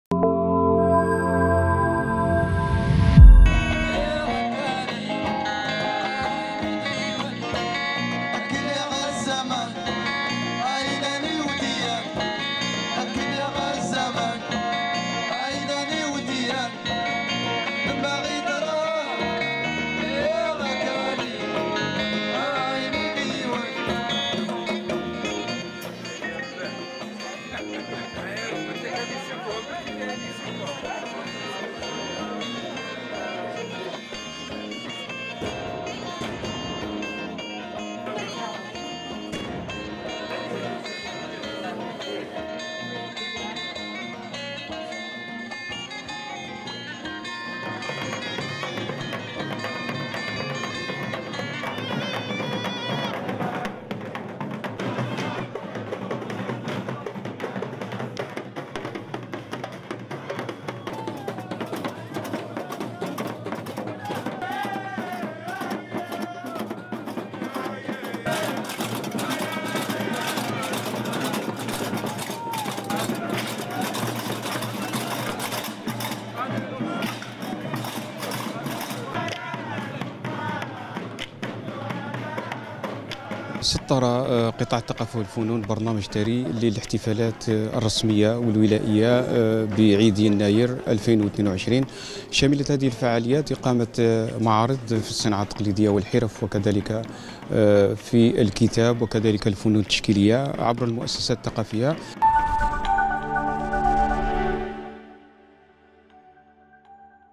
ورقلة : احتفالات يناير.. فرصة لتثمين الموروث الثقافي الأمازيغي
ورقلة (وأج) - تشكل الاحتفالات برأس السنة الأمازيغية  مناسبة لتثمين الموروث الثقافي الأمازيغي, حيث تتجدد فيه الصلة بموروث الأجداد من عادات وتقاليد وبالتراث الثقافي الأصيل.